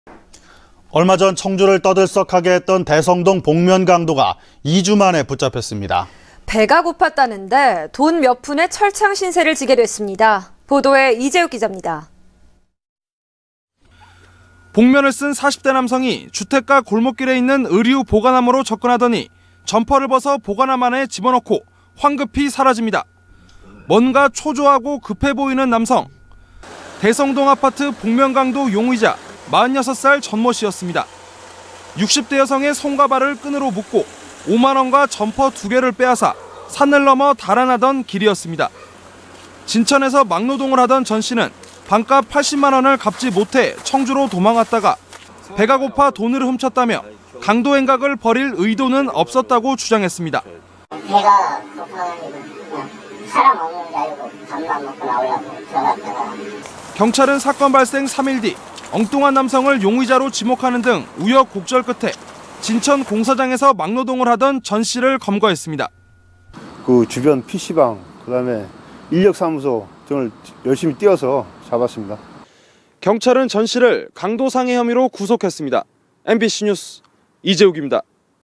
[청주MBC뉴스] 배고픈 도둑 복면강도로 돌변
◀앵커▶
 연초 청주를 떠들썩하게 했던 대성동
복면강도가 2주만에 붙잡혔습니다.
배가 고팠다는 데 돈 몇푼에 철장 신세를
지게 됐습니다. 이재욱 기자입니다.
           ◀ＥＮＤ▶

           ◀ＶＣＲ▶
 복면을 쓴 40대 남성이 주택가 골목길에 있는
의류보관함으로 접근하더니 점퍼를 벗어
보관함 안에 집어넣고 황급히 사라집니다.

 뭔가 초조하고 급해보이는 남성.

 대성동 아파트 복면강도 용의자
46살 전 모 씨였습니다.

 60대 여성의 손과 발을 끈으로 묶고 5만원과 점퍼 2개를 빼앗아 산을 넘어 달아나던
길이었습니다.

 진천에서 막노동을 하던 전 씨는
방값 80만 원을 갚지 못해 청주로 도망왔다가
배가 고파 돈을 훔쳤다며 강도 행각을
벌일 의도는 없었다고 주장했습니다.

           ◀ＳＹＮ▶ 강도 용의자 전 씨
    "배 고파서 밥 먹으러 갔다가 그만"

 경찰은 사건 발생 3일 뒤 엉뚱한 남성을
용의자로 지목하는 등 우여곡절 끝에
진천 공사장에서 막노동을 하던 전 씨를
검거했습니다.

           ◀ＩＮＴ▶ 전성민/상당서
    "인력사무소 돌아다니며 탐문해 검거"

 (s/u) 경찰은 전 씨를 강도상해 혐의로
구속했습니다. MBC뉴스 이재욱입니다.
           ◀ＥＮＤ▶